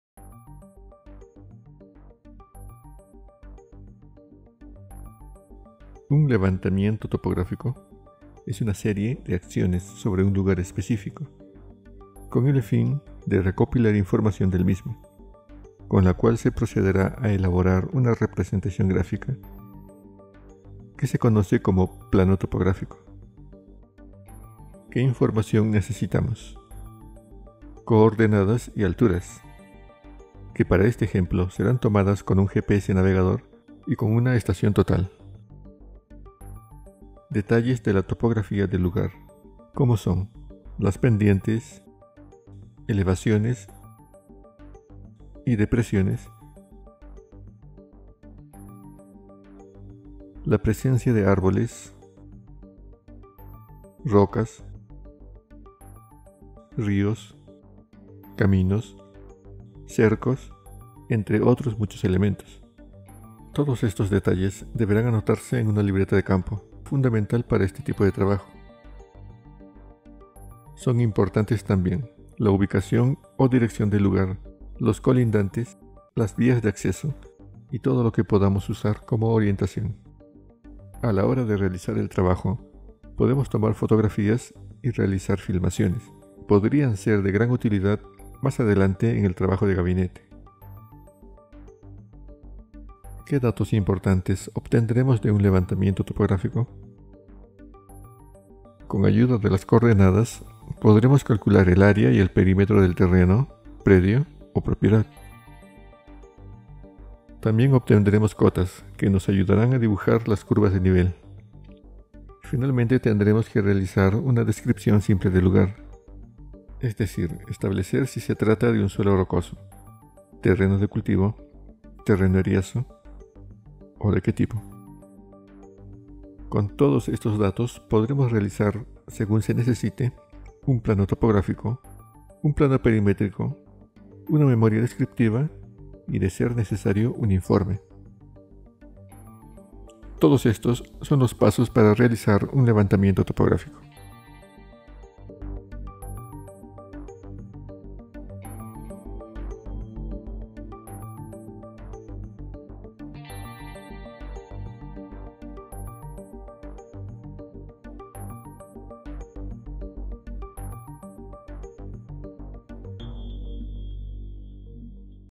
Levantamiento topográfico - Definición (02/20)
Un levantamiento topográfico es una serie de acciones sobre un lugar específico, con el fin de recopilar información del mismo. Con la cual se procederá a elaborar una representación gráfica, que se conoce como plano topográfico.

Levantamiento topográfico - Definición